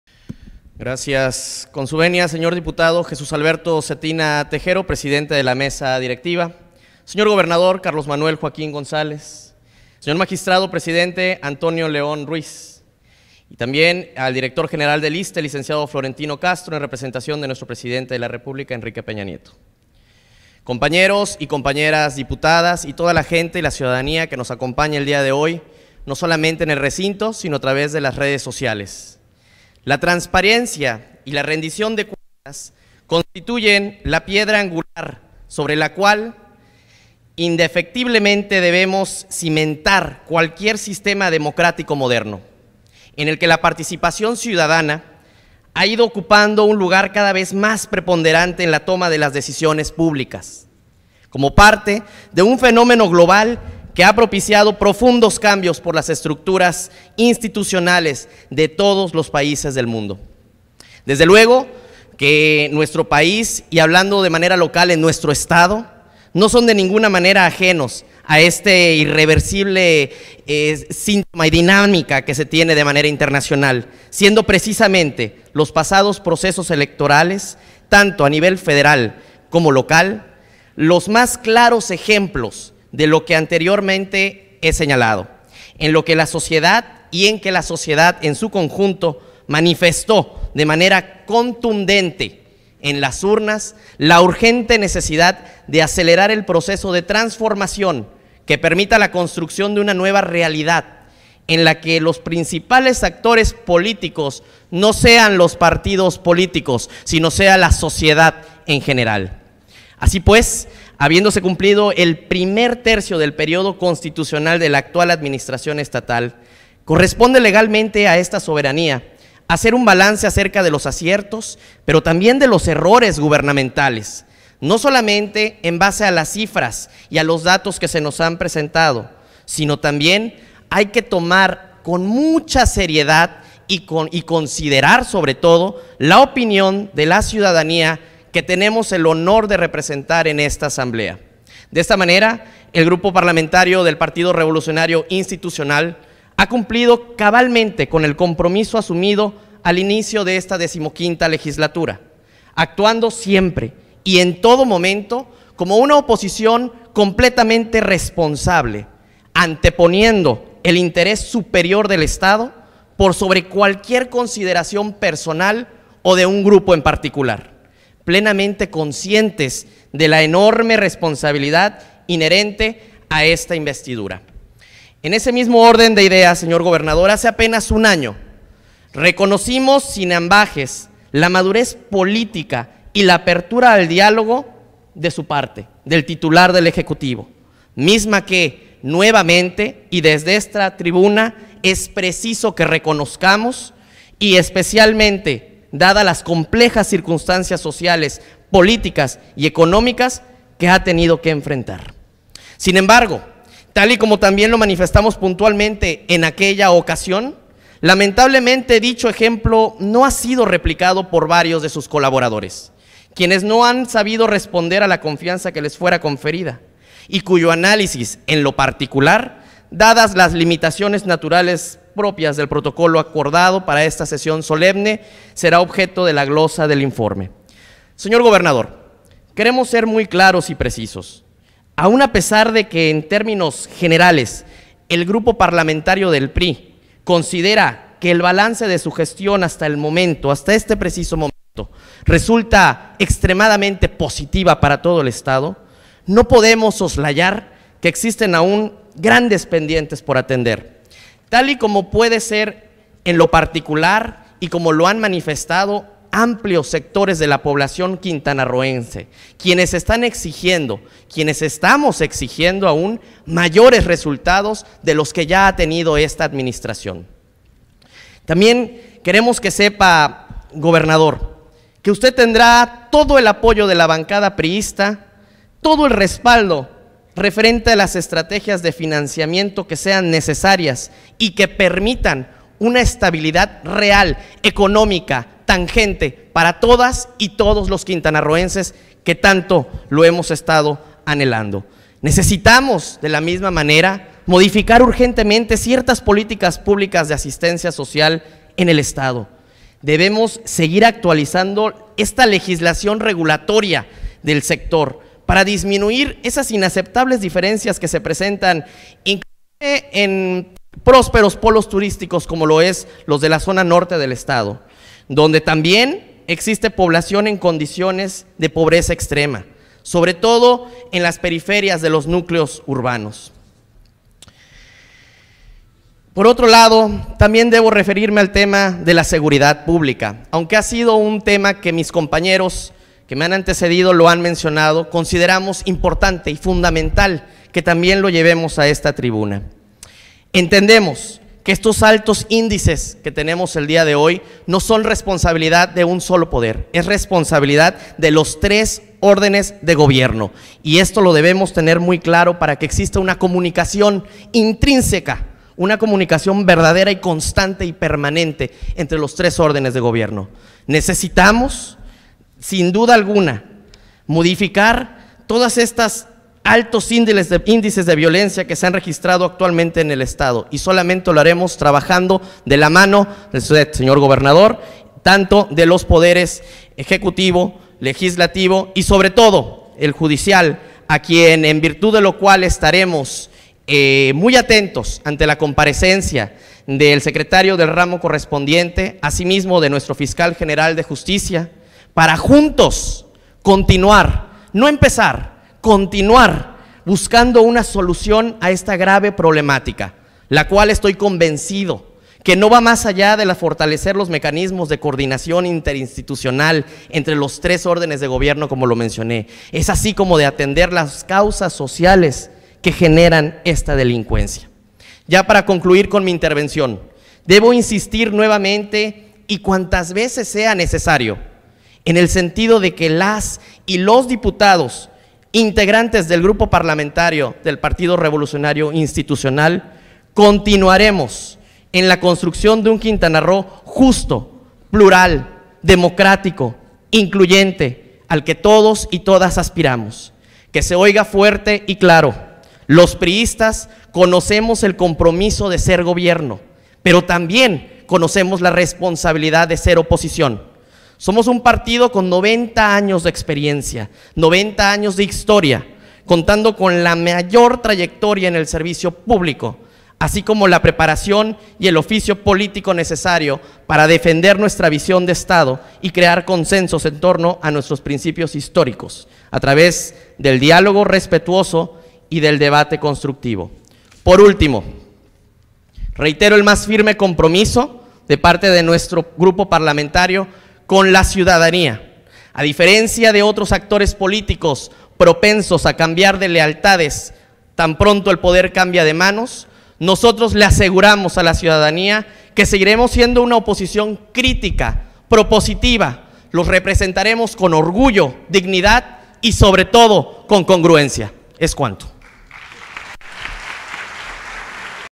Intervención del Dip. Alberto Vado Morales. PRI (8/Sep/2018)
Intervención del Dip. Alberto Vado Morales, en representación del Partido Revolucionario Institucional, en relación al Segundo Informe del Titular del Poder Ejecutivo, sobre el estado que guarda la Administración Pública Estatal.